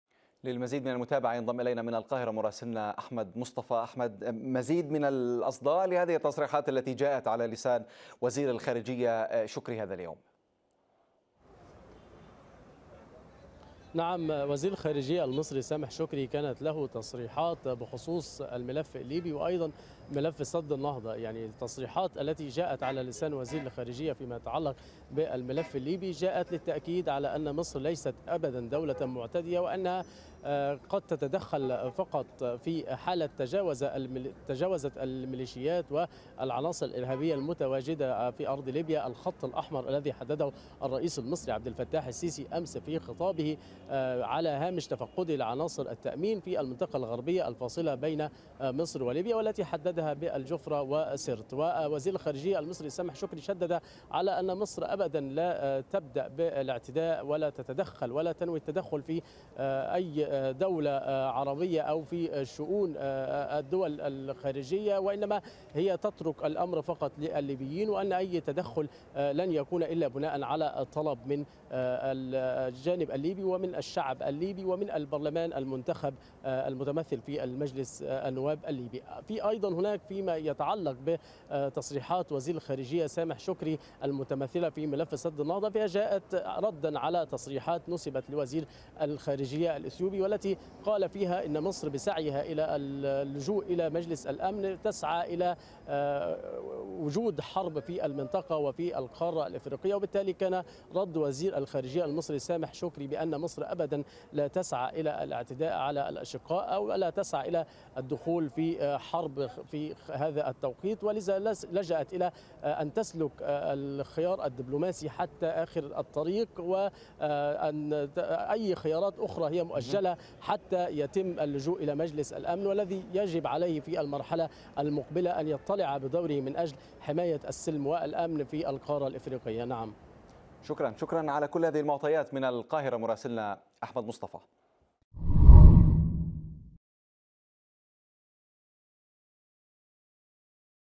رسائل قوية من الخارجية المصرية بشأن الملفين الليبي وأزمة سد النهضة.. تعرف عليها
صرح السفير حسام زكى الأمين العام المساعد بجامعة الدول العربية بأن الأمانة العامة تلقت طلباً من مصر لعقد اجتماع طارىء لمجلس الجامعة على مستوى وزراء الخارجية لبحث  تطورات الأوضاع فى ليبيا. وأشار الأمين المساعد إلى أنه يجرى التنسيق حاليا لتحديد موعد الاجتماع.
مزيد من التفاصيل مع مراسل الغد في القاهرة  أحمد مصطفى.